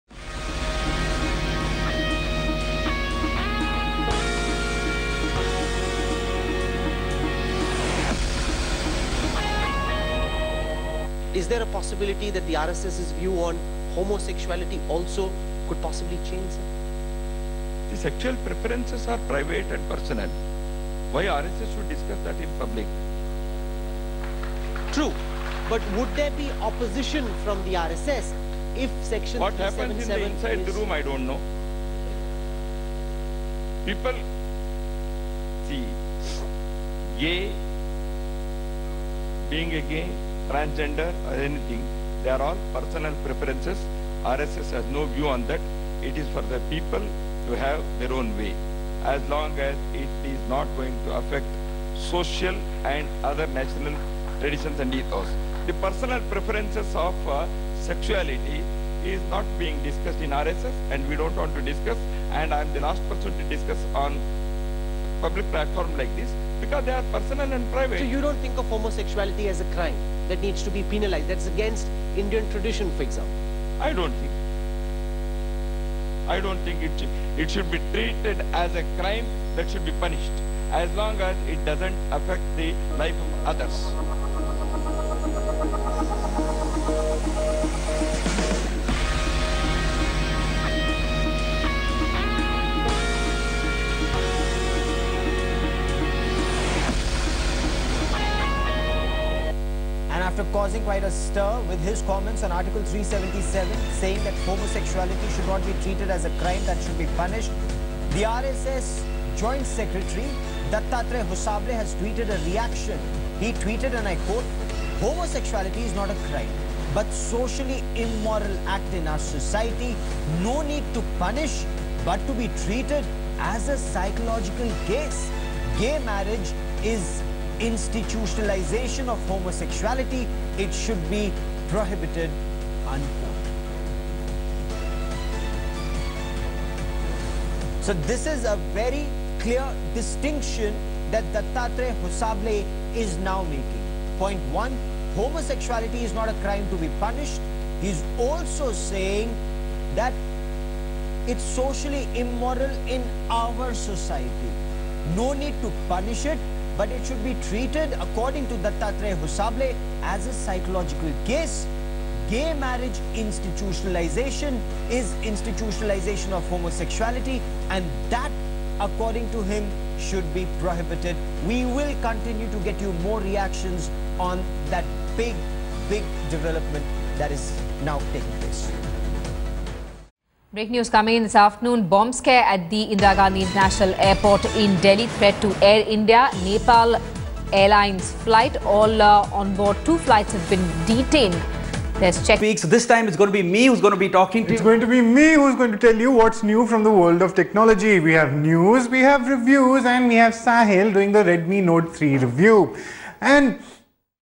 Homosexuality Not A Crime : RSS Joint Secretary, Dattatreya Hosabale At India Today Conclave 2016
RSS Joint Secretery Dattatreya Hosabale says homosexuality is not a crime at India Today Conclave 2016.